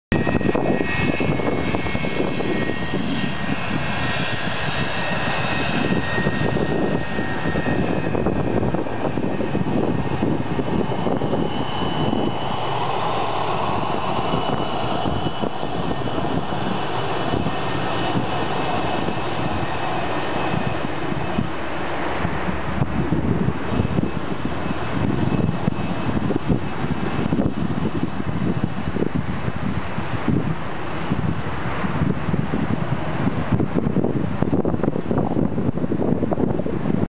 Aeroparque - SABE - MD-88 - Austral Lineas Aereas (Ex AeroMexico)
Uno de los 3 MD-88 Ex AeroMexico, ahora de Austral Lineas Aereas, matricula LV-BTI, llendo a la cabecera de la pista 13 del Aeroparque Jorge Newbery.